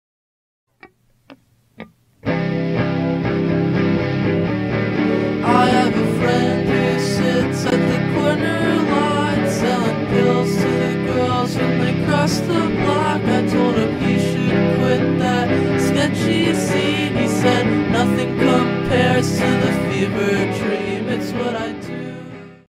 [] I walked past a stranger today [] Hazbin Hotel AU [] Ft. Adam and Fiora [] Guitar siblings angst?
Characters used
Exorcist leader! Fiora
Sinner! Adam

Screenshots taken
8